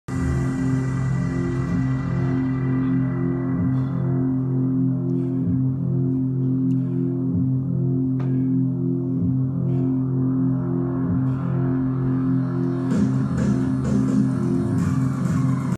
Static Lunges / Split Squats
More single sided exercises, spiltsquats or staticlunges if you prefer 😁.